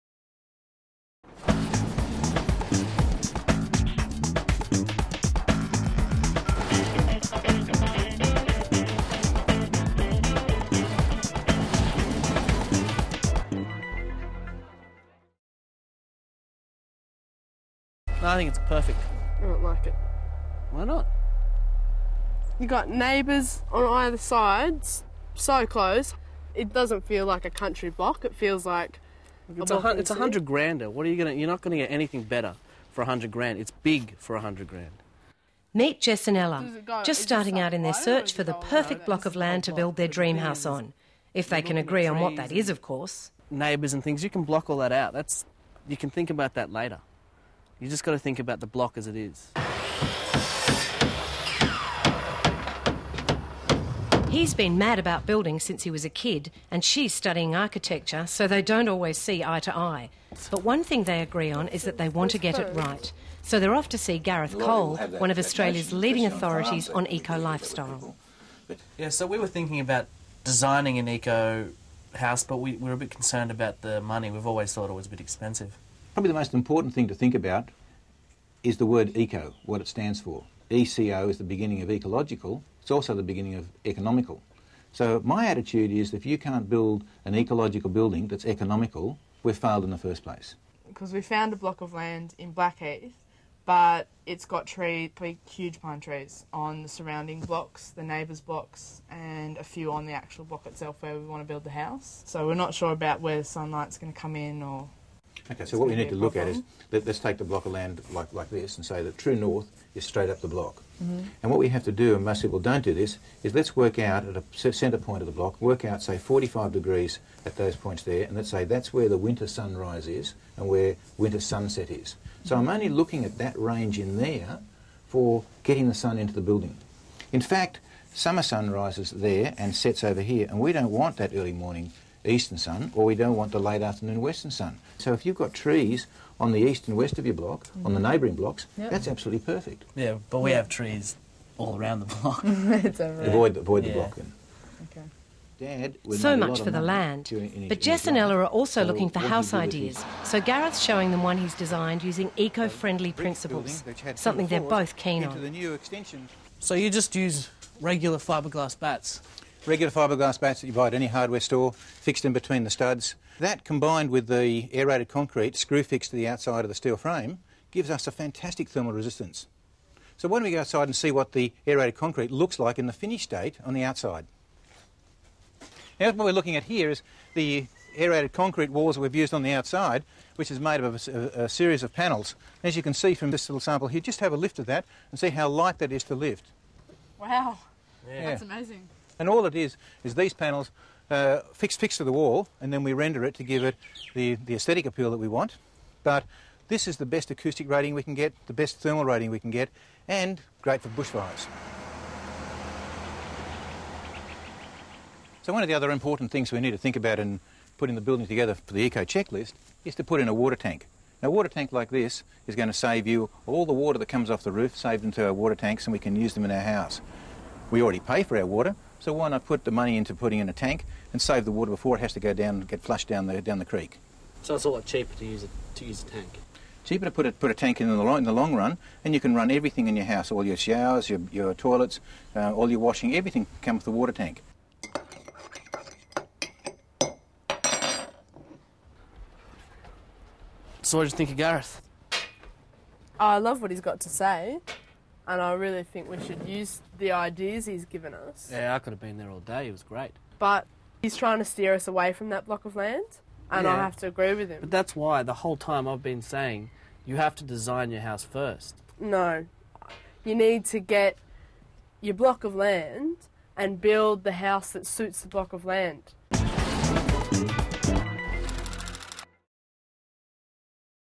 Eco House Episode 1
Eco House is an on line TV show that follows the journey of a young couple's quest to build a sustainable home in the Blue Mountains of NSW Australia.